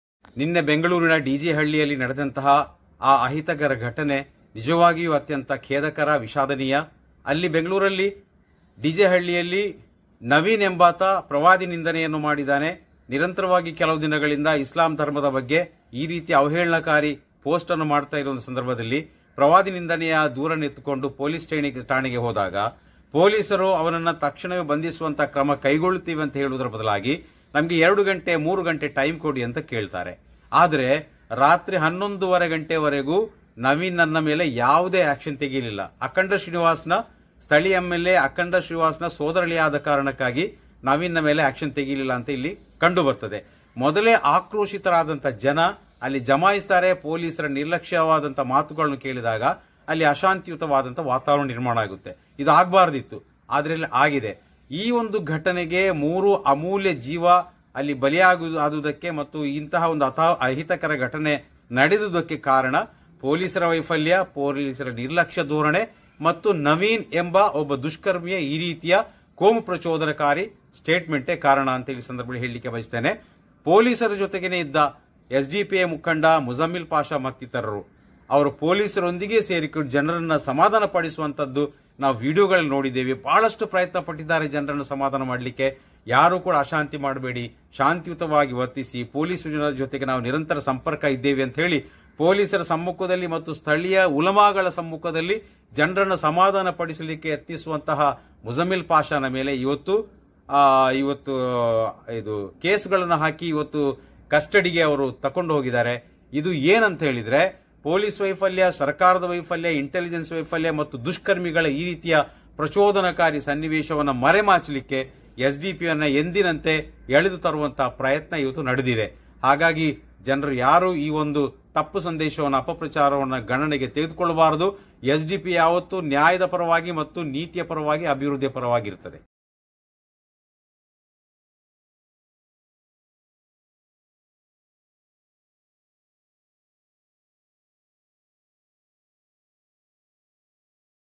ಬೆಂಗಳೂರು ಗಲಭೆಗೆ ಸ್ಥಳೀಯ ಪೊಲೀಸರ ನಿರ್ಲಕ್ಷ್ಯ ಕಾರಣ - ಎಸ್ ಡಿಪಿಐ
ಬೆಂಗಳೂರಿನ ಡಿಜೆ ಹಳ್ಳಿಯಲ್ಲಿ ಗಲಭೆಗೆ ಸ್ಥಳೀಯ ಪೊಲೀಸರ ನಿರ್ಲಕ್ಷವೇ ಕಾರಣ ಎಂದು ಎಸ್ ಡಿಪಿಐ ಆರೋಪ ಮಾಡಿದೆ. ಈ ಕುರಿತಂತೆ ಮಾಧ್ಯಮಗಳಿಗೆ ಹೇಳಿಕೆ ಬಿಡುಗಡೆ ಮಾಡಿರುವ SDPI ರಾಜ್ಯಾಧ್ಯಕ್ಷ ಇಲ್ಯಾಸ್ ಮೊಹಮ್ಮದ್ ತುಂಬೆ, ನವೀನ್ ಎಂಬಾತ ಪ್ರವಾದಿ ಹಾಗೂ ಇಸ್ಲಾಂ ಧರ್ಮದ ಬಗ್ಗೆ ಅವಹೇಳನ ಸಾಮಾಜಿಕ ಜಾಲತಾಣದಲ್ಲಿ ಅವಹೇಳನ ಮಾಡಿದ್ದರು.